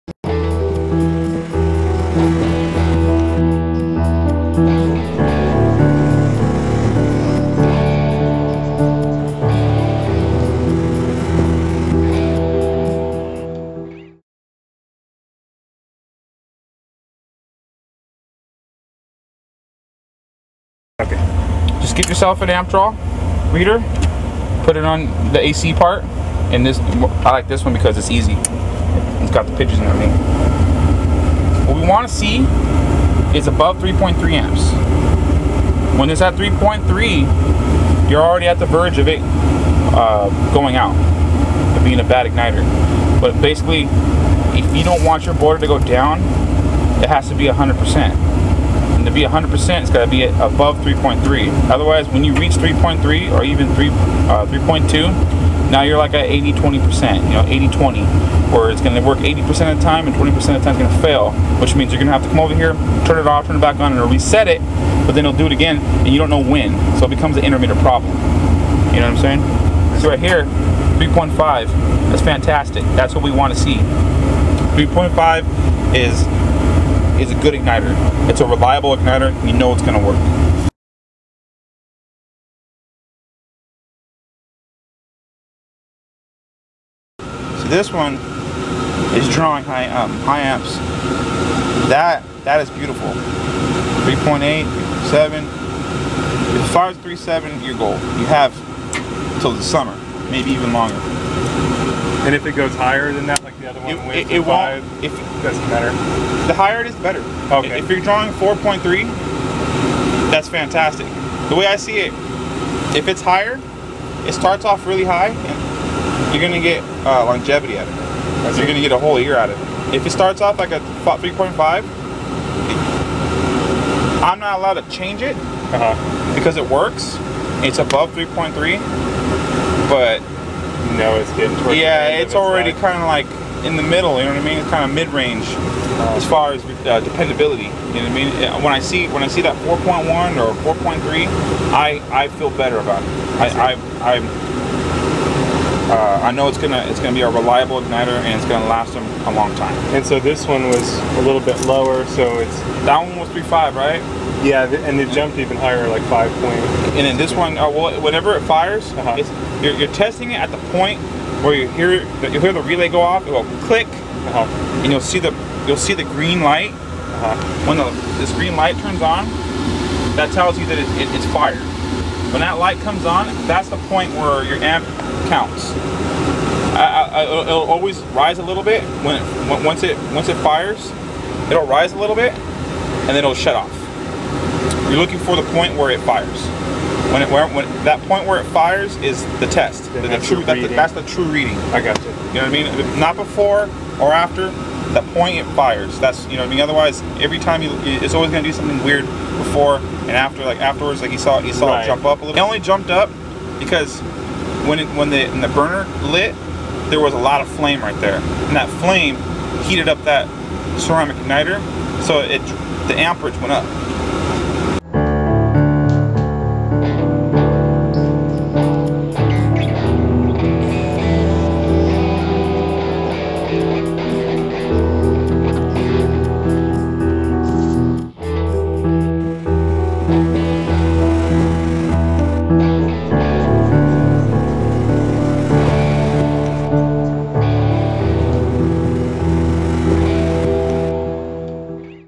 HVAC Boilers How To Test Boiler Igniters For Proper Amp Draw
Kung Fu Maintenance and a friend test two boiler igniters on a two stage setup for proper amp draw.